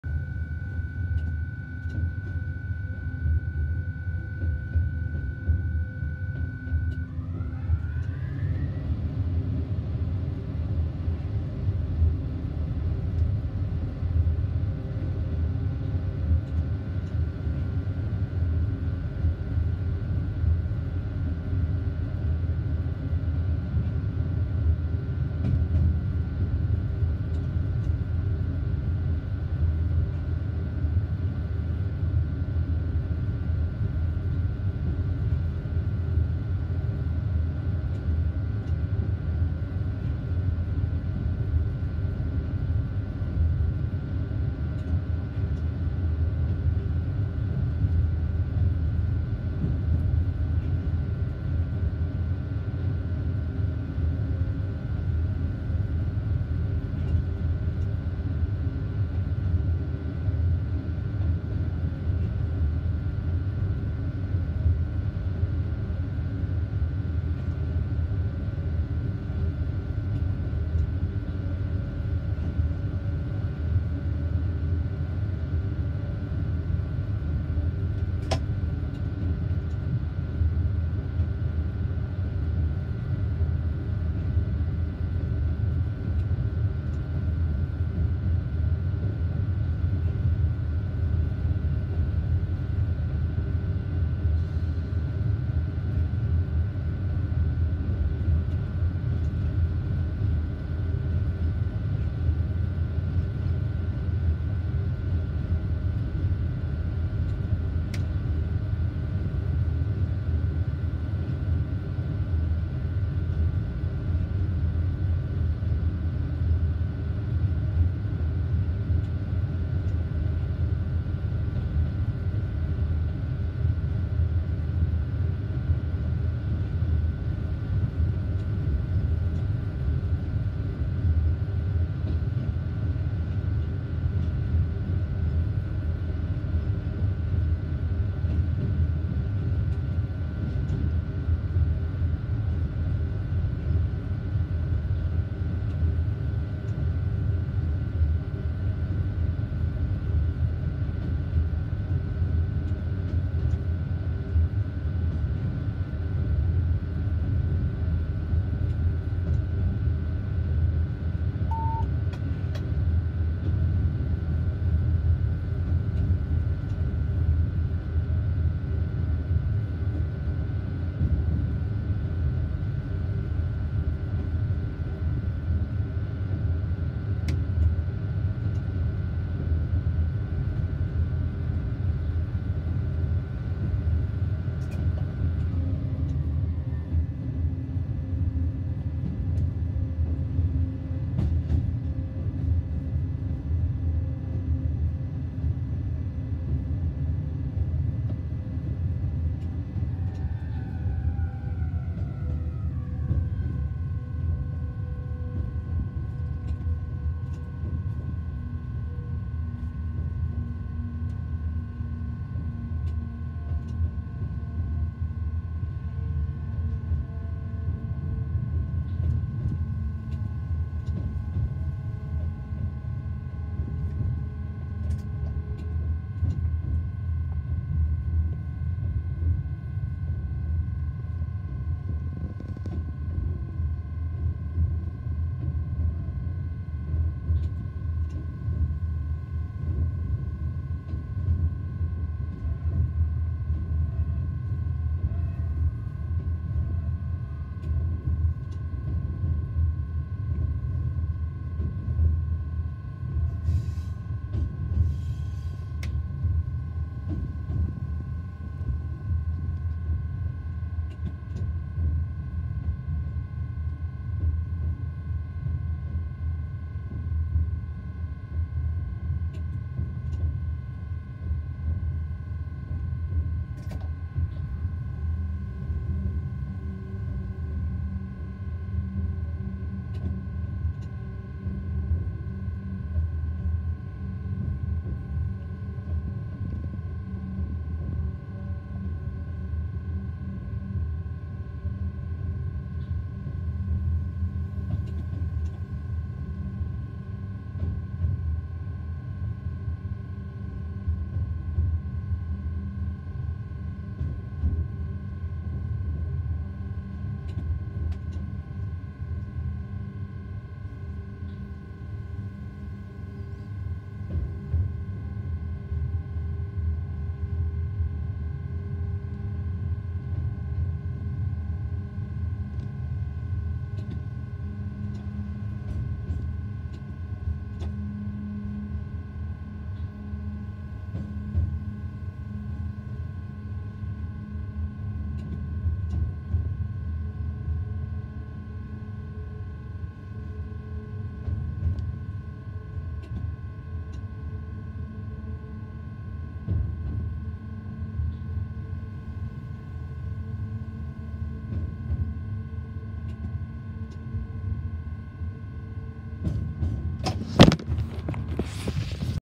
BB 27000 zone de Dunkerque.
Sortie du  faisceau Colombier dans la zone de Dunkerque.